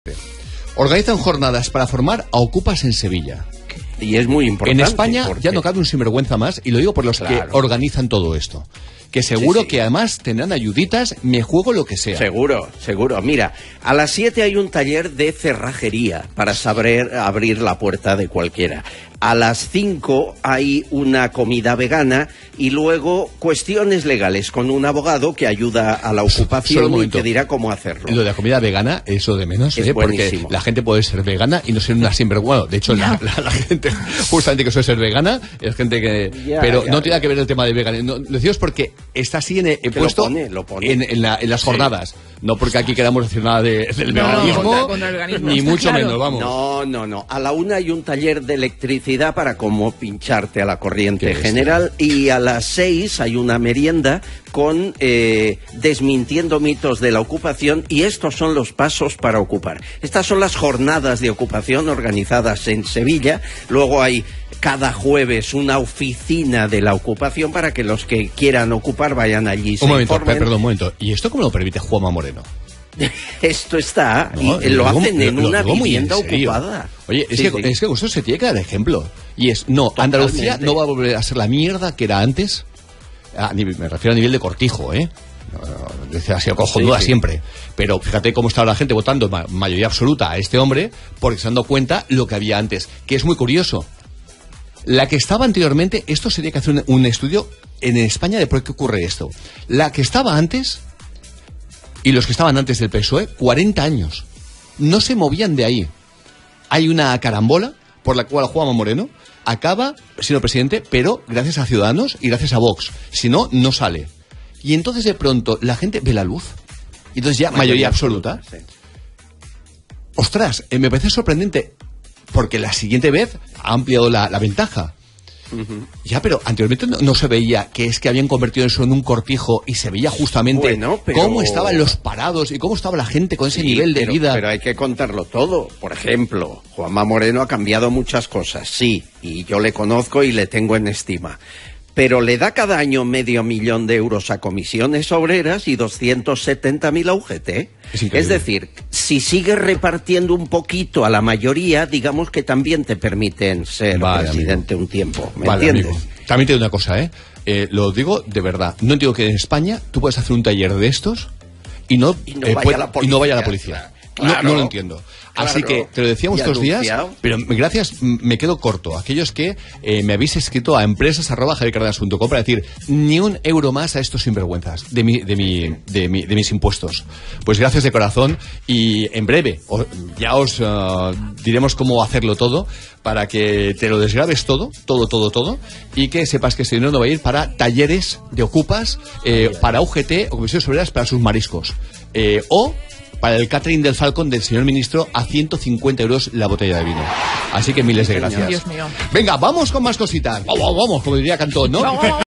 JAVIER CÁRDENAS 🔥ACOJONANTE🔥 Se organizan jornadas para formar a okupas en Sevilla
➡ ¿Me ayudas a crear más contenido?:

Tienda de AMAZON